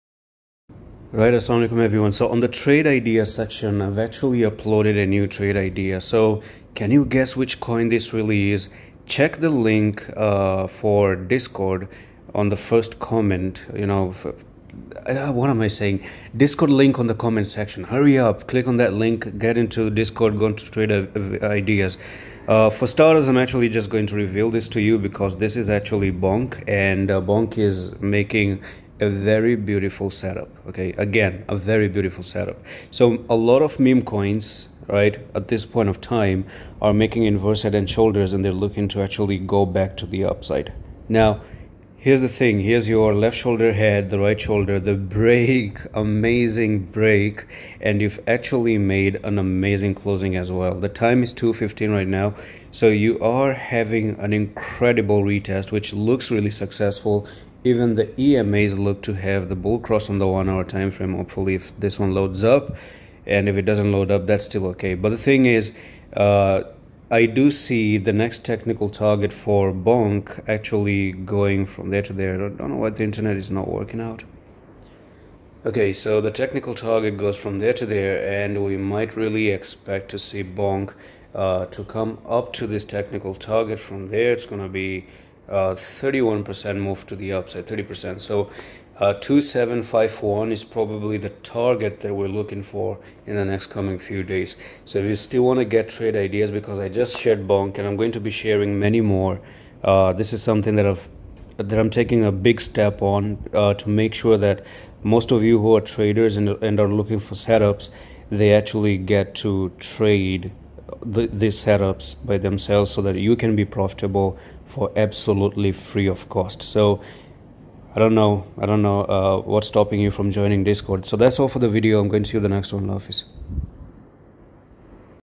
BONK Coin (BONK) Price Prediction and Technical Analysis, A BEAUTIFUL SETUP !
BONK COIN update today for 4 AUGUST 2024